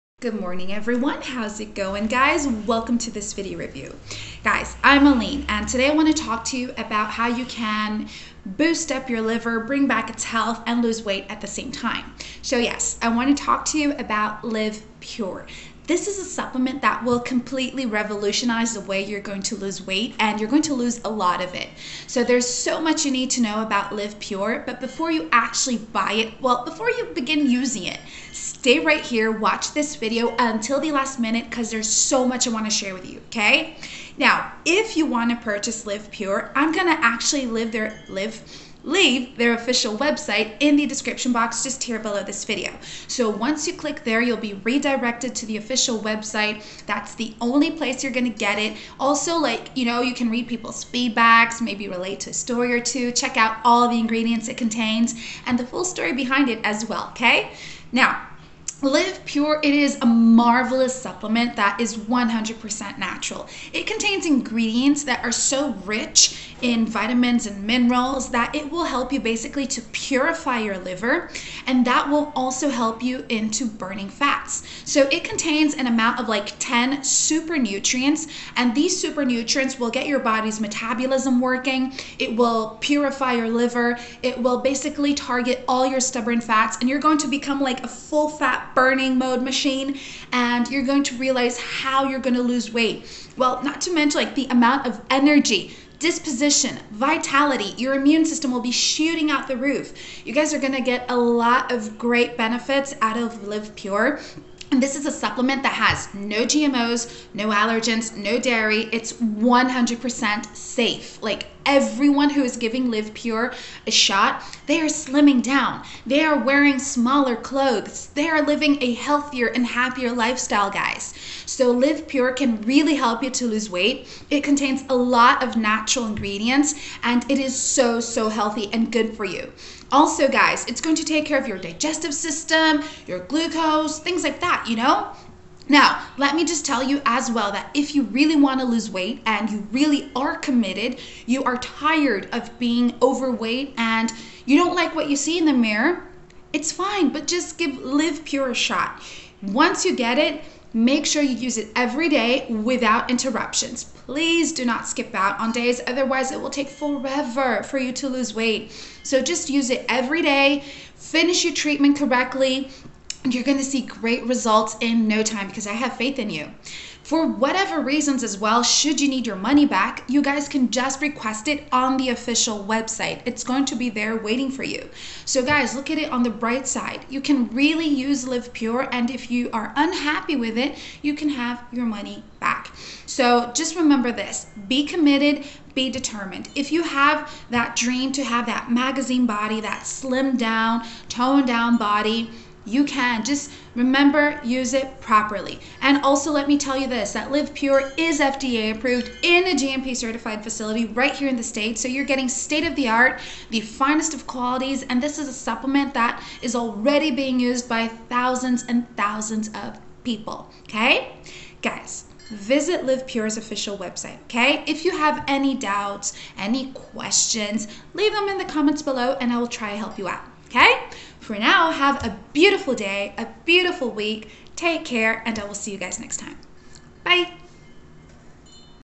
LIV PURE - LIVE PURE REVIEW (⚠️BE VERY CAREFUL!❗️❌) LIVPURE - LIVPURE REVIEWS - Liv Pure Supplement
Liv Pure Review - Achieve Natural and Safe Weight Loss

📌 Liv Pure Review – Liv Pure Reviews
Introducing Liv Pure, a dietary supplement that goes beyond targeting belly fat and unhealthy weight gain. This powerful formula addresses the underlying cause of excess abdominal fat by promoting a natural and healthy weight loss process. Rigorous research and clinical trials have unveiled that an excessive buildup of ceramides in the bloodstream can contribute to the accumulation of fat around vital organs such as the liver, heart, and arteries. By incorporating Liv Pure into your routine, you can effectively combat this issue and discover a comprehensive solution to cleanse your body, optimize your liver health and liver detoxification.

📌 How Does Liv Pure Work? The first blend is the liver purification complex, which is specifically designed to cleanse and detoxify the body. This complex aims to eliminate toxins and promote a healthy liver function.
The second blend is the liver-fat burning complex, which is formulated to boost metabolism and target stubborn body fat. By enhancing metabolic processes, Liv Pure aims to facilitate faster fat and calorie burning, ultimately supporting natural weight loss.

📌 Liv Pure Ingredients: The Liver Purification Complex contains ingredients that helps to detoxify and regenerate the liver. Let's take a closer look at some of the key components:

• Sylimarin:  Studies have shown the effects of Sylimarin in reducing inflammation and promoting cell regeneration.

• Betaine: Betaine helps with liver function and cell regeneration. Supplementation of Betaine prevents and treats fatty liver and preserves liver function in cases of liver injury.

• Berberine: Berberine is a chemical found in several plants. Research suggests that Berberine helps to promote weight loss , enhance blood sugar, and treats several medical conditions. This ingredient can inhibit oxidative stress and inflammation in the liver.

• Molybdenum: Molybdenum is a chemical element that is sued to make alloys. This essential trace mineral is a key compo

📌 Benefits of Liv Pure: Liv Pure is a weight loss supplement that claims to offer several benefits to its users. Here are some of the potential benefits of taking Liv Pure:
• Helps to optimize your liver health.
• Supports liver detoxification.
• Helps to prevent unhealthy inflammation.
• Reignites metabolism.
• Speed up calorie burning
• Cleanse your body
• Supports cognitive function

📌 Is Liv Pure safe?
Liv Pure is made using the finest quality ingredients that are 100% natural. It does not contain soy, dairy, or GMOs. It's manufactured in an FDA-approved and GMP-certified facility using state-of-the-art equipment under a strict and sterile environment in the US. The supplement also undergoes third-party inspections for quality standards.

📌 Liv Pure Guarantee – Liv Pure Where to Buy: You can actually test Liv Pure out for 60 Days, If you don't see results or if you don't like it for any reason they'll give you your money back. So it's completely risk-free .
You will only find Liv Pure on the official website. So take advantage of this offer today by clicking the link below to secure your discount!

Important Moments of This Video:
00:00 – Liv Pure Review
00:15 – Liv Pure What is
00:45 – Liv Pure Official Website
01:00 – Liv Pure Ingredients
02:10 – Liv Pure Benefits
02:30 – Liv Pure How to Take
02:50 – Liv Pure Guarantee